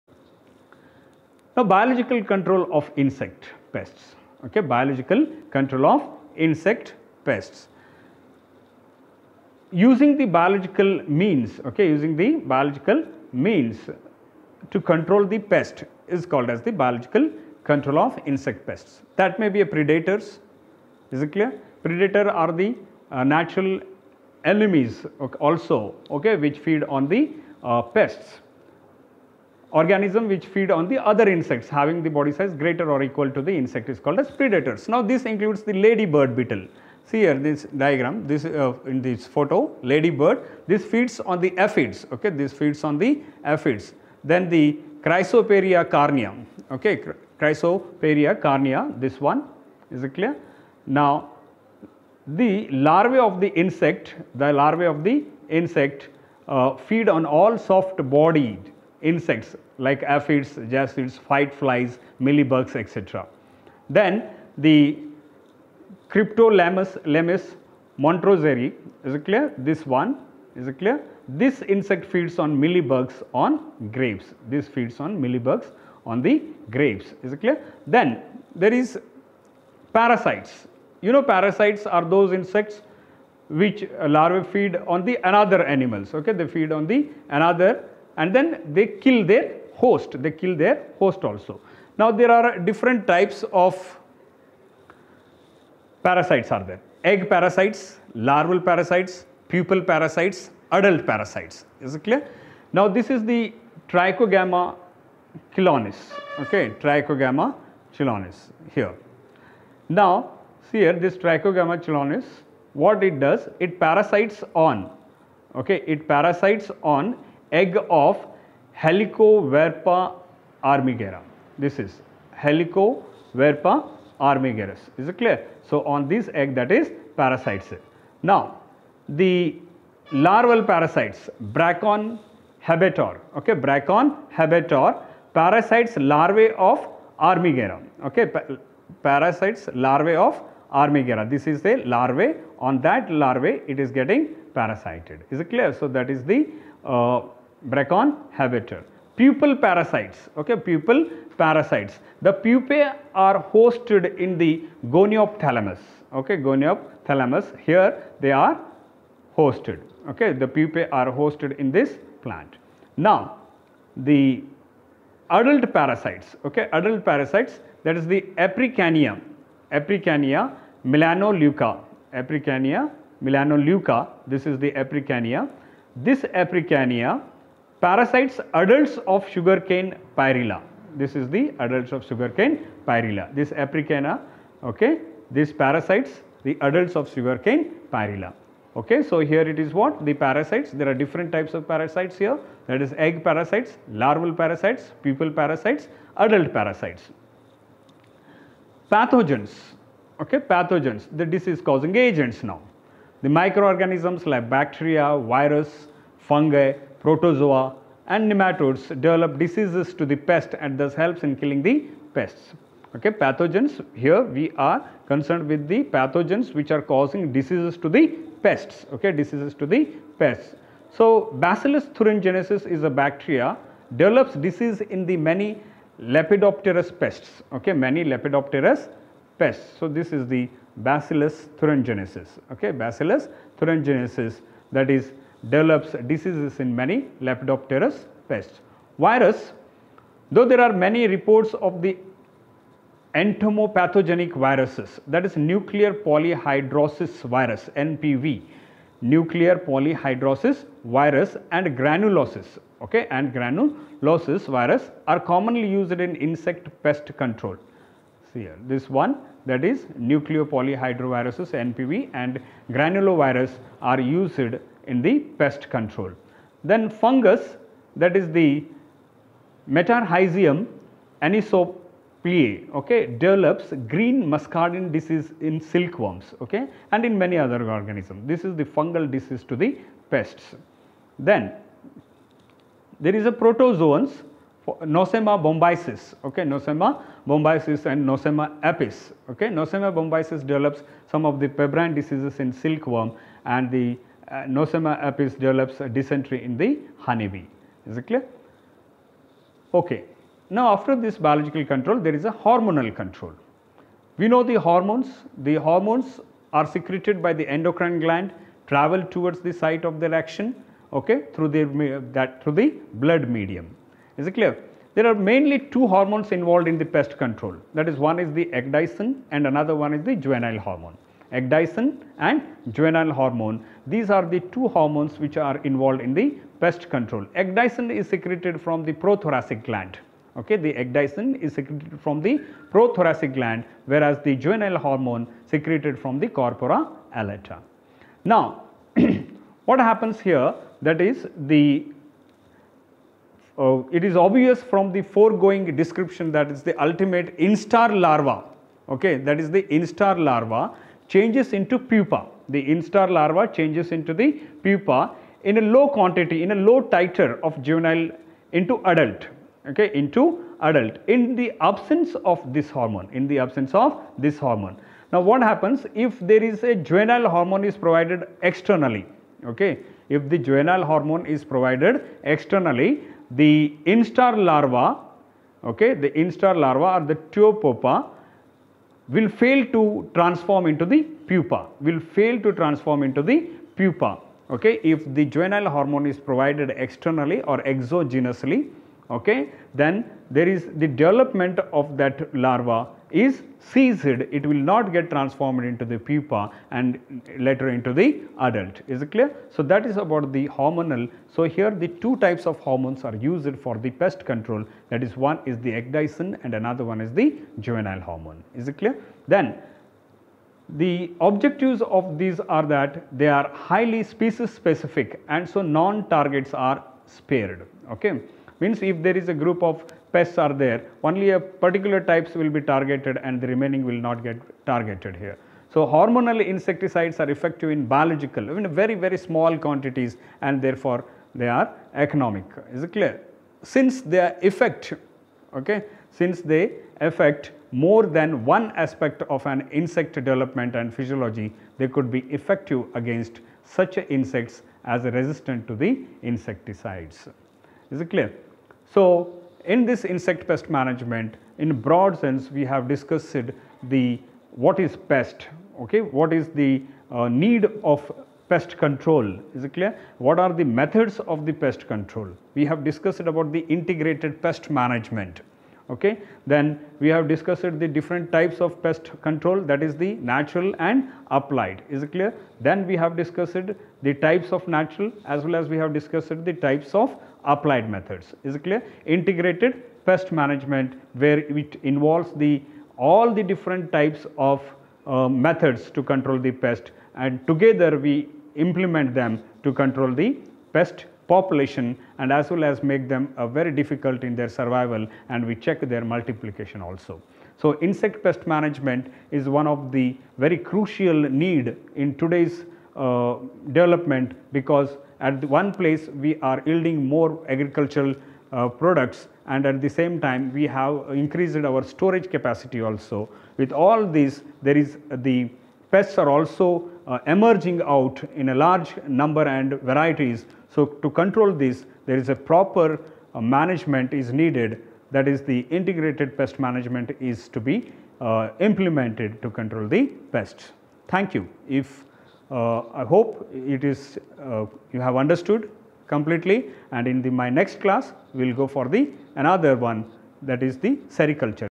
B. Sc. Zoology.. INSECT PEST MANAGEMENT PART 3
Integrated Pest Management, Applied Method, Biological Pest Control Methods and Hormonal Pest Control Methods..